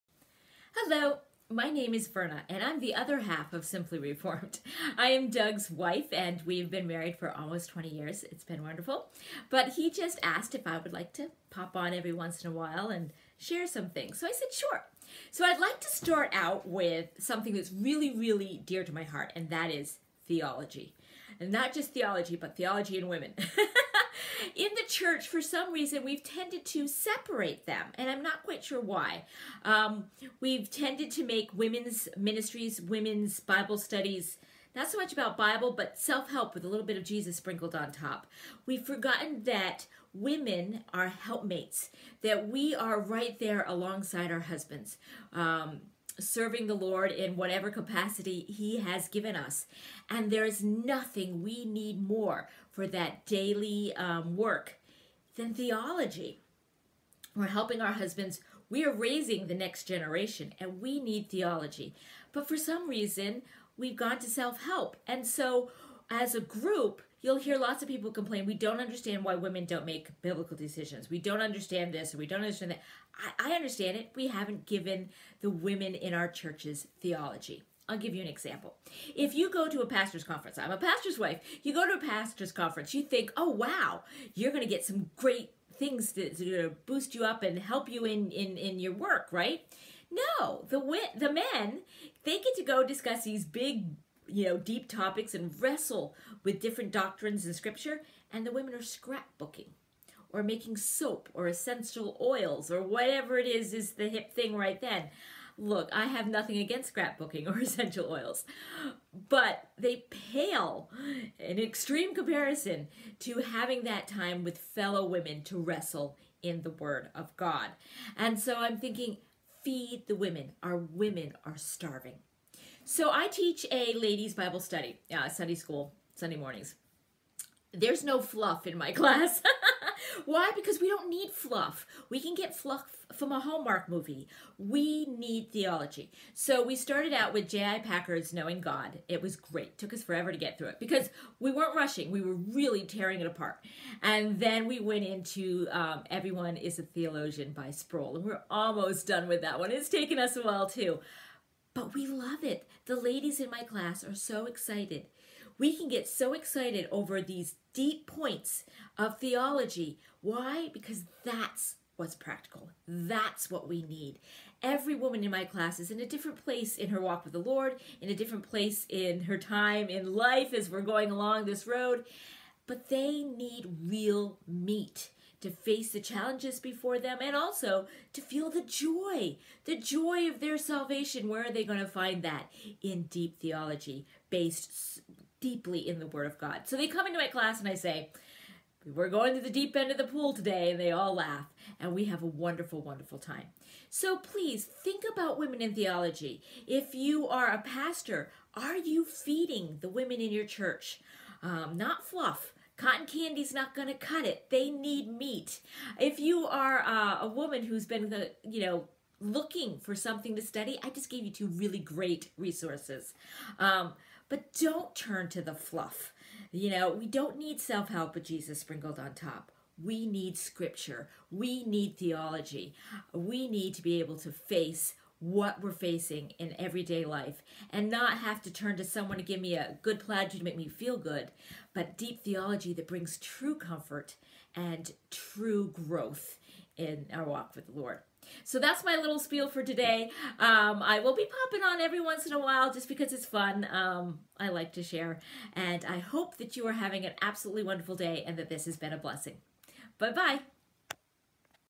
Woman & Theology?
Should women do theology?
My lovely wife, Verna, takes on one of her favorite topics.